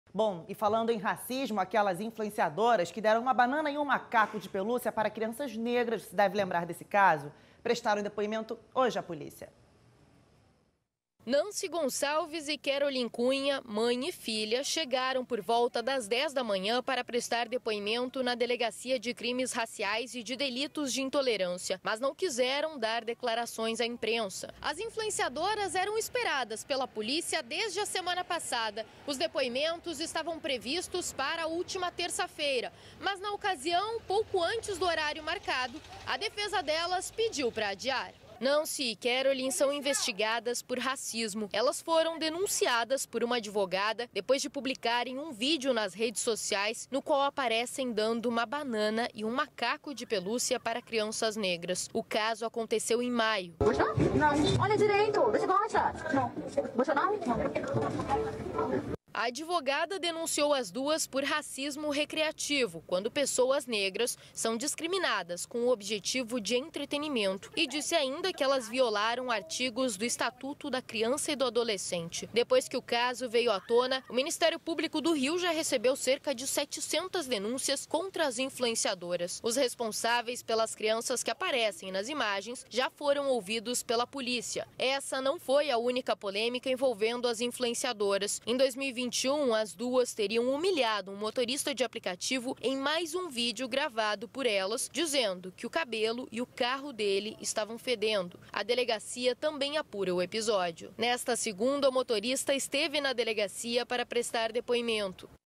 Racismo: influenciadoras prestam depoimento na delegacia
As influenciadoras acusadas de racismo que deram uma banana e um macaco de pelúcia para crianças negras prestaram depoimento hoje na delegacia.